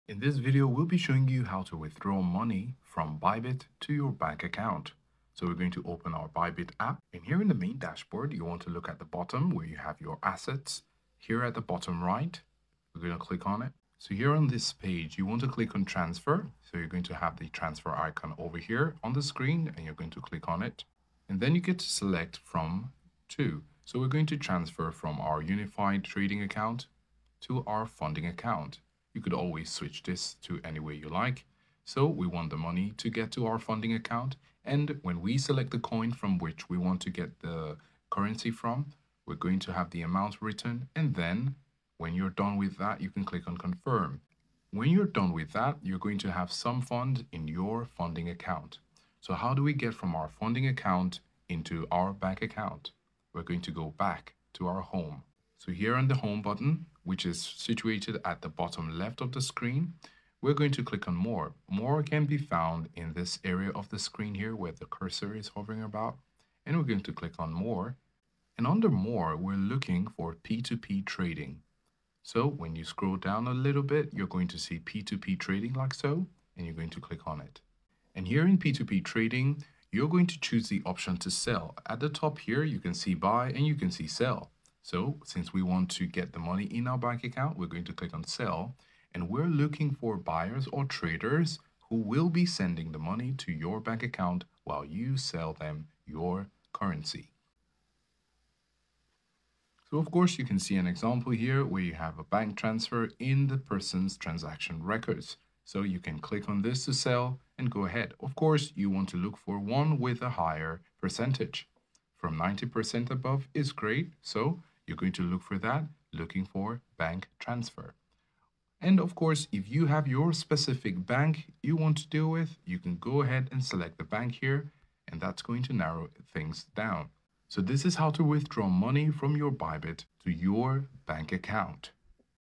How to Withdraw Money From Bybit to Bank Account - Quick Tutorial - 2024
How to Withdraw Money From Bybit to Bank Account. Quick Step-By-Step tutorial.

✅ Sign up for a Bybit account and claim up to 6,045 USDT bonus (💲 Use Code: RDLWB)